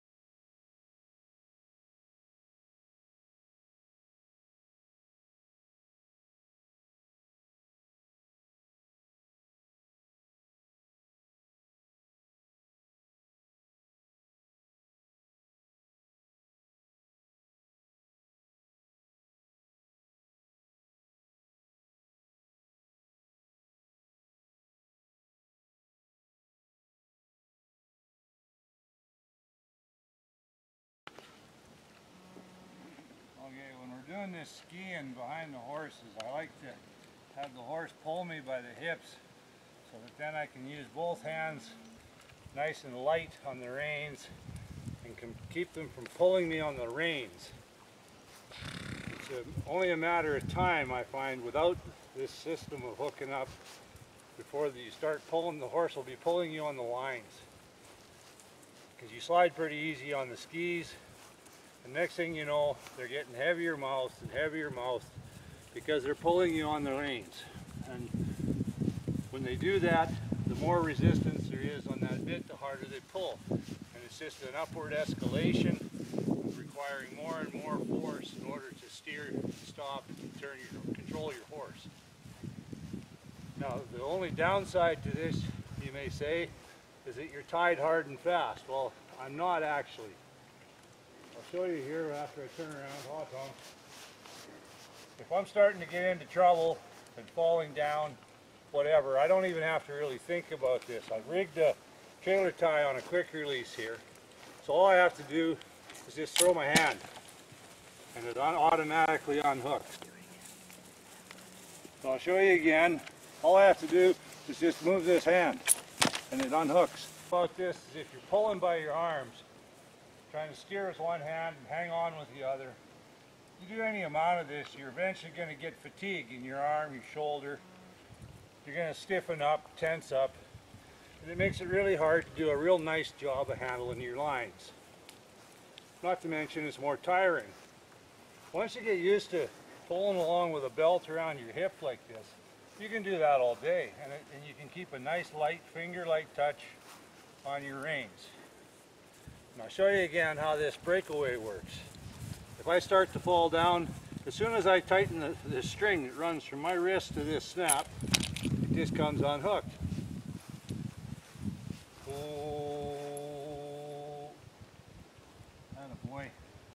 Miniature Horse Skijoring
A introductory look at our skijoring harness, shafts, and including Kelly's thoughts on doing this! Skijoring can be fun, but it does inherently contain elements of danger that anyone, with any style of rigging MUST be cognizant of!